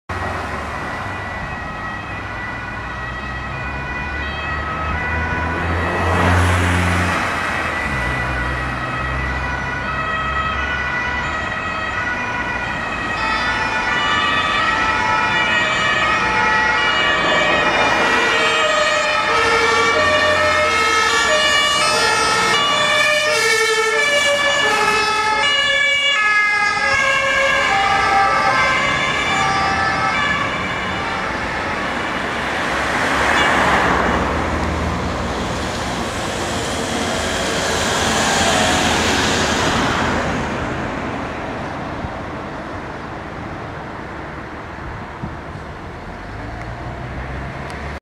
RTW 83/1 JUH + NEF JUH RW Heidenau in Pirna
Ambulance 83/1 and the emergency doctor car from the St. John's EMS station Heidenau responding in Pirna.

Pirna, Saxony, Germany, 5/2011.

Der Rettungswagen 83/1 und das Notarzteinsatzfahrzeug der Johanniter Rettungswache Heidenau auf Einsatzfahrt in Pirna.

Pirna, Sachsen, Deutschland, 5.2011.